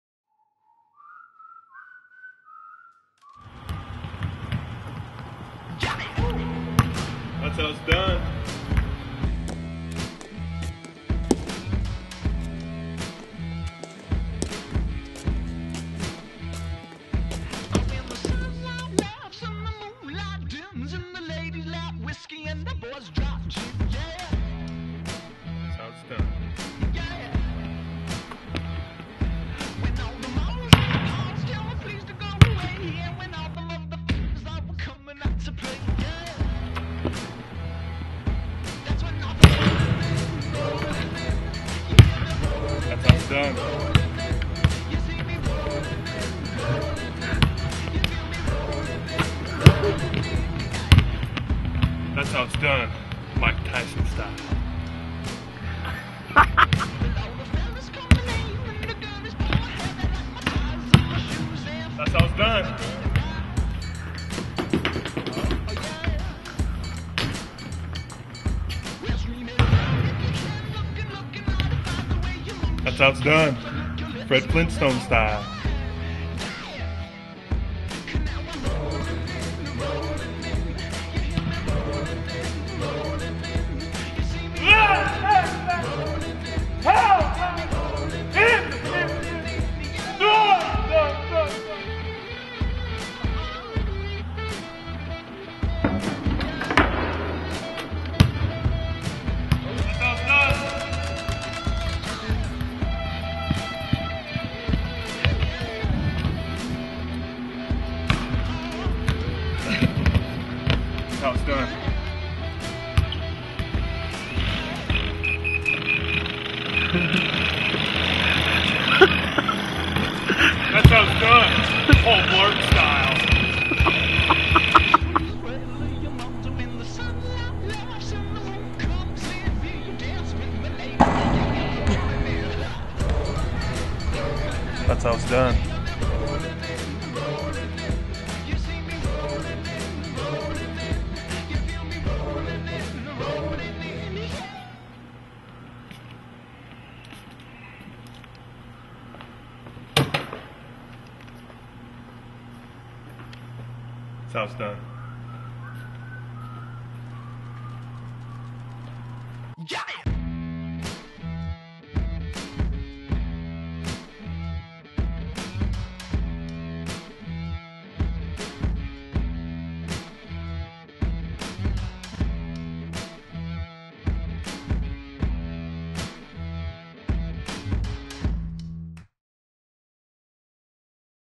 I canestri assurdi di Jon Brockman all'Harris Bradley Center di Milwaukee
Jon Brockman compie canestri ai limiti della finzione. That's how it's done!